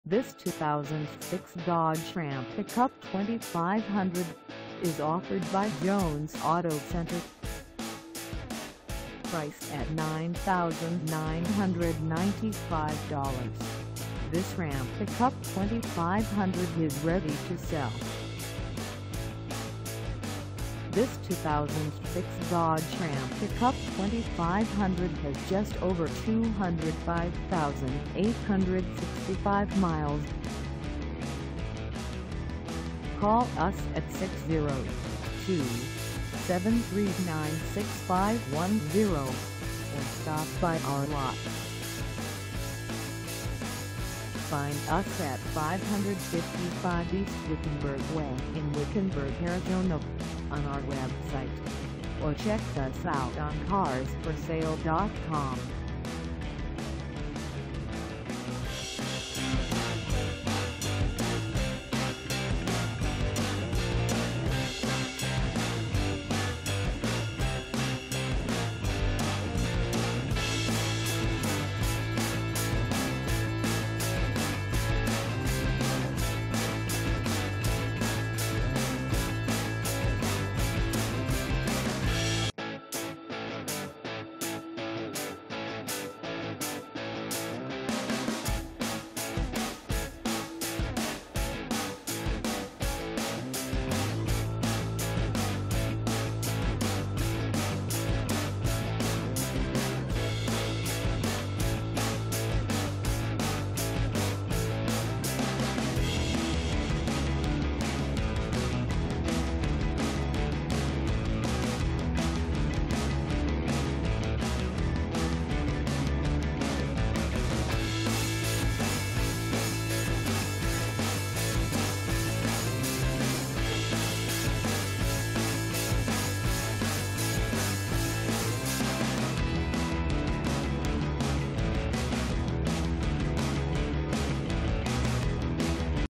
2006 Dodge Ram Pickup 2500 2dr Reg Cab 140.5 ST for sale in
This 2006 Dodge Ram Pickup 2500 2dr Reg Cab 140.5 ST is for sale in Wickenburg, AZ 85390 at Jones Auto Center.

Engine: 5.9L I6
Transmission: AUTOMATIC
Color: Bright White
Mileage: 205865
Contact Jones Auto Center 555 E Wickenburg Way Wickenburg, AZ 85390 to test drive this 2006 Dodge Ram Pickup 2500 2dr Reg Cab 140.5 ST today.